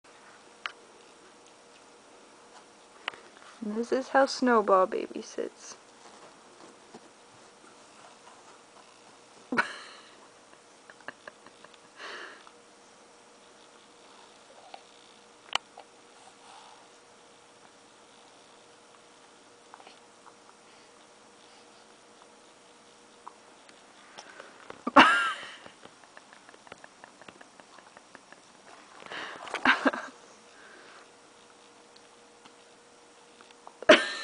Puppy Nursemaid
This baby has this puppy wrapped around her little finger! While Snowball seems to find it funny the bite and tackle the rest of us, baby Elena has only ever gotten sweet kisses and cuddles. Lucky baby!